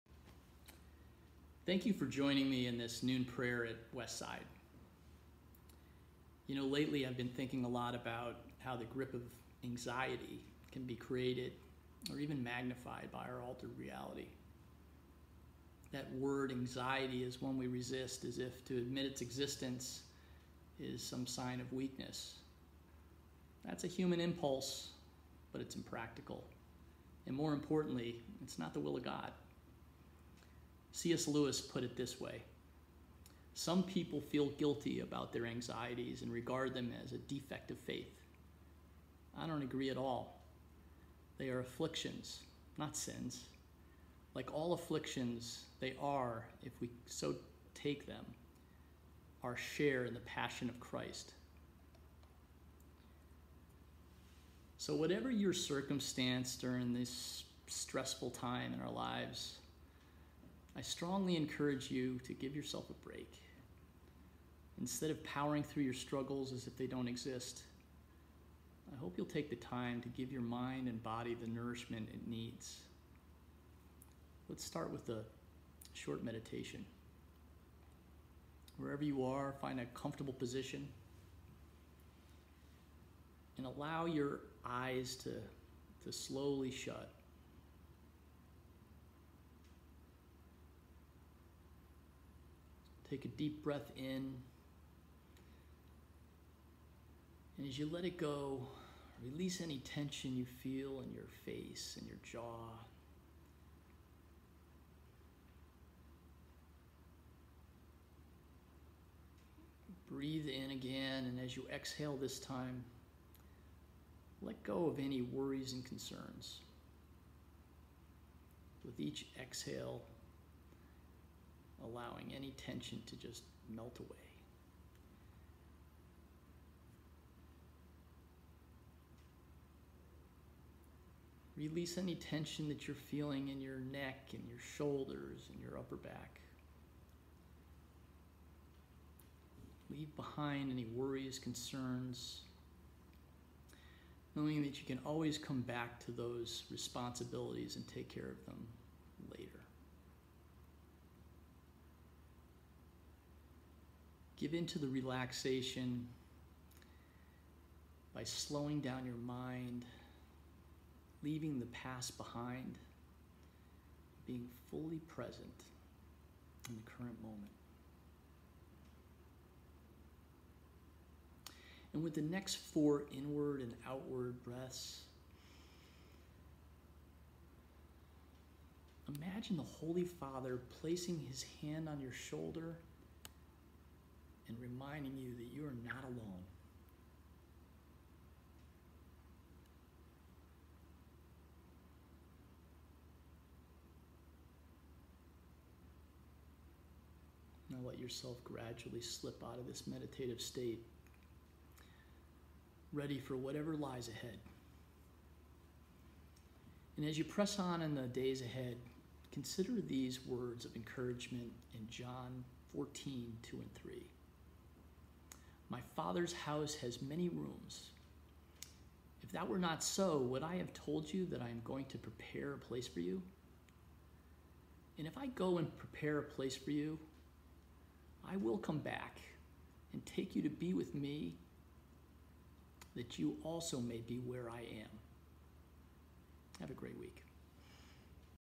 Noon Prayer with Jim Roche
Whatever your circumstance during these stressful times, give yourself a break! Take some time to give your mind and body the nourishment it needs. Member Jim Roche leads us in a short meditation.